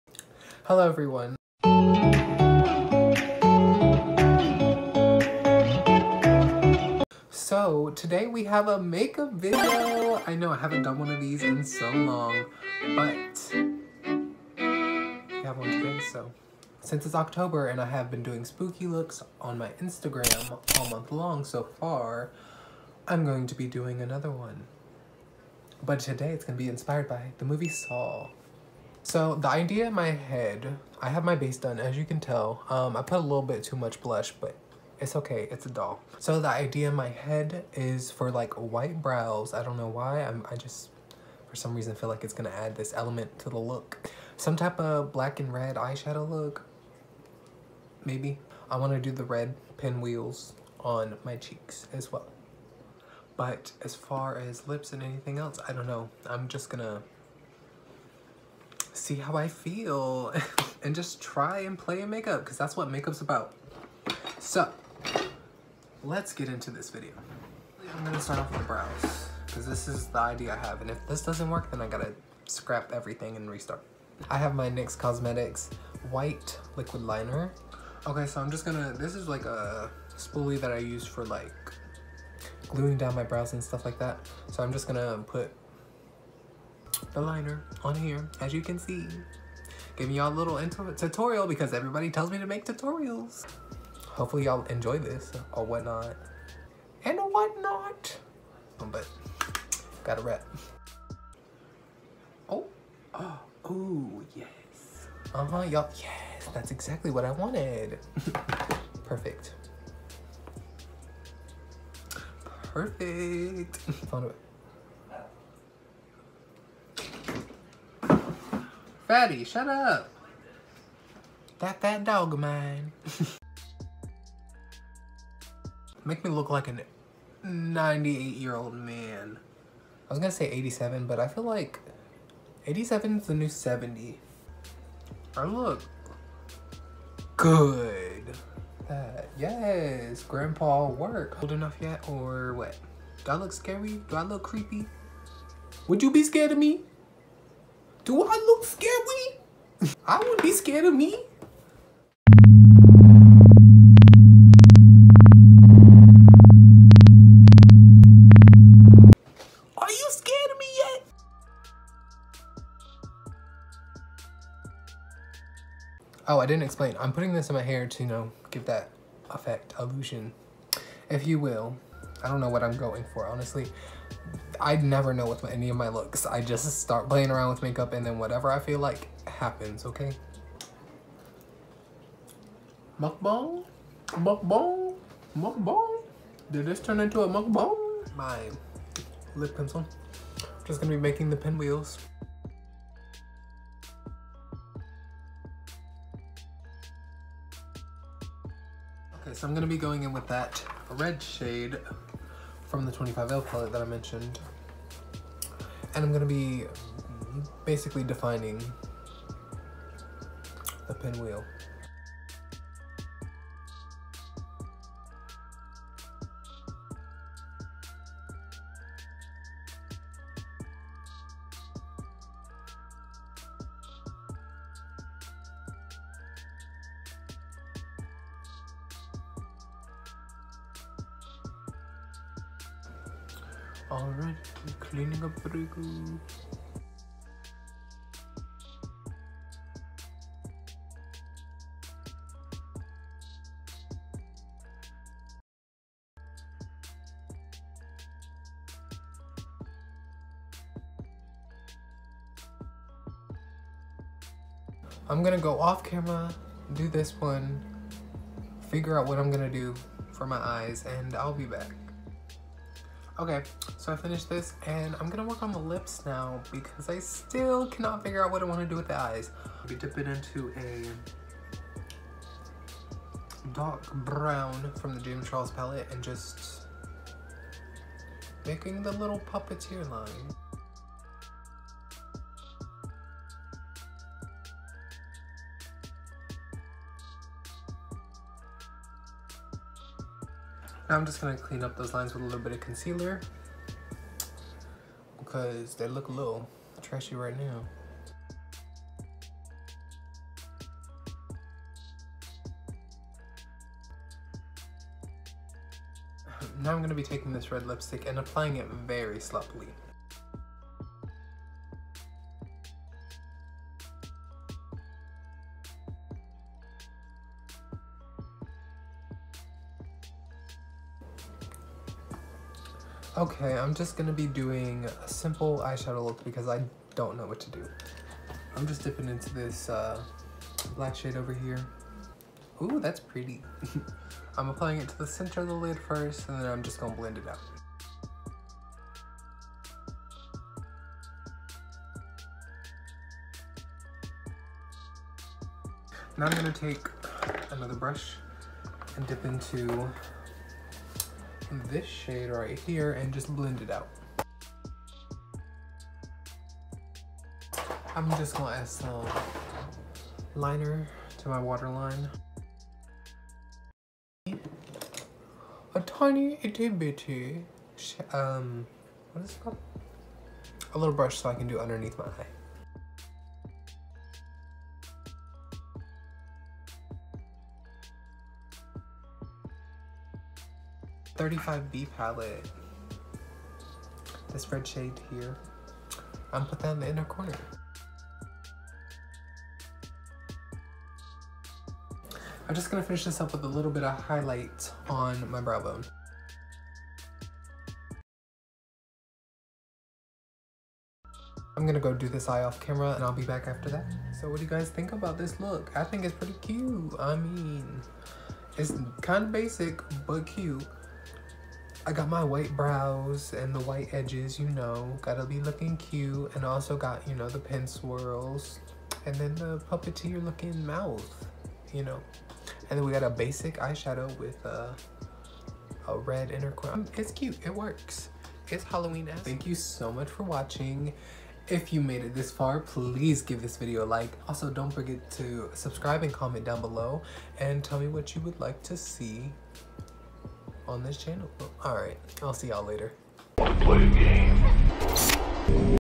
SAW INSPIRED HALLOWEEN MAKEUP TUTORIAL!
This is just a little makeup tutorial I filmed for you guys I felt inspired by the movie saw , I hope I did it justice lol !

Products used💋⠀
* 25L palette /James Charles Palette
* ABH dip brow/ brow wiz
* Lily lashes ( so extra Miami )
* Supreme frost (wet dream) ⠀
*kush mascara
*i am magic foundation/concealer
*the sculptor palette/ bronze and brighten